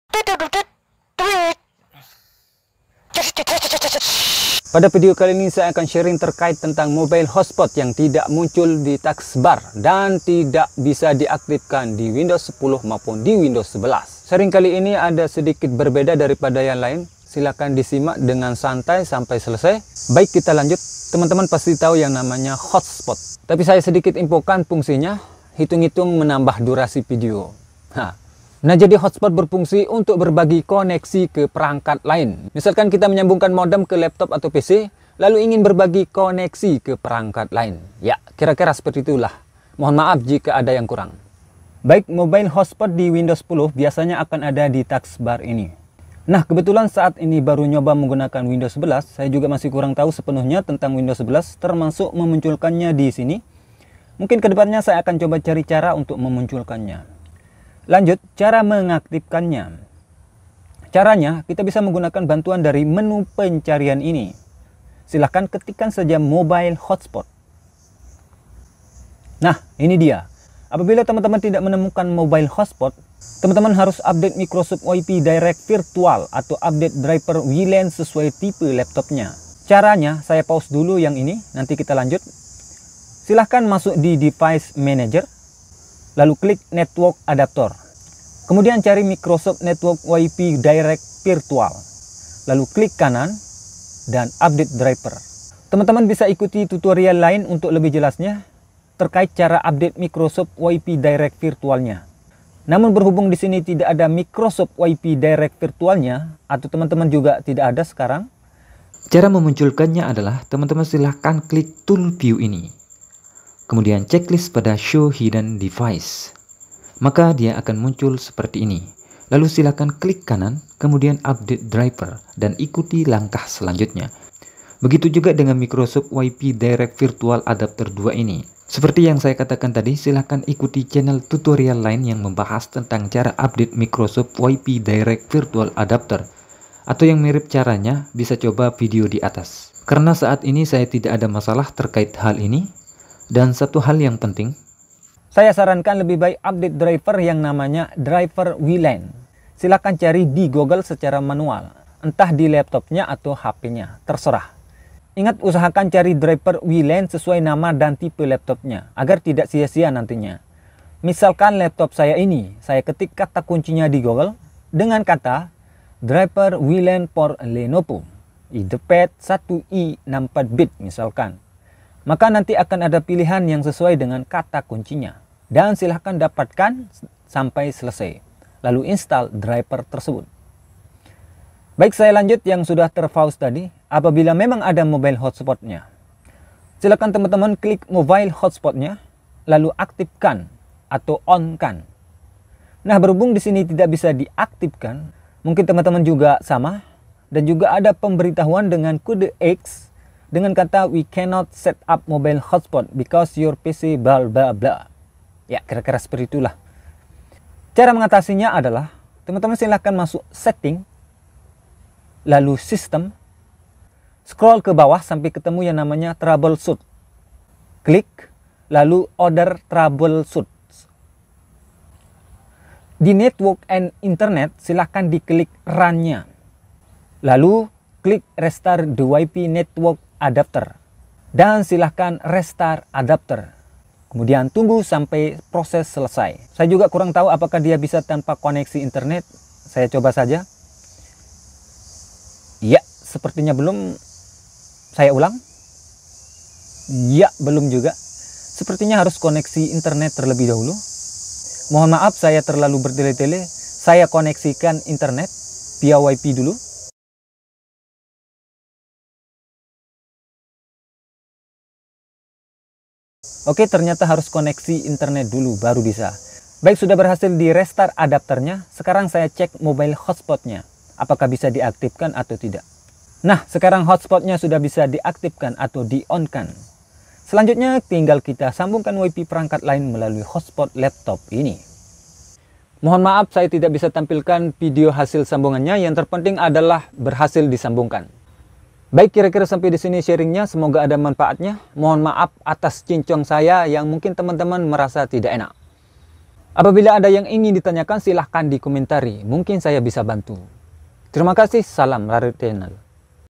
Mengatasi Mobile Hotspot yang tidak bisa diaktifkan ||Memunculkan Microsoft Wifi Direct Virtual Adp.
Pada Video kali ini, saya sharing bagaimana cara mengatasi mobile hotspot di Windows 10/11 yang tidak bisa diaktifkan, sekaligus cara memunculkan Microsoft Wifi Direct Virtual Adapter di Network Adapter_Device Manager.

Dalam video ini saya berikan dua solusi, pastinya berbeda dari pada yang lain dengan Tutorial lain. 98% pasti berhasil.

Silahkan disimak dengan santai sampai selesai!
Semoga bermanfaat!
Mohon maaf atas ketidak nyamanan teman-teman! Terima kasih.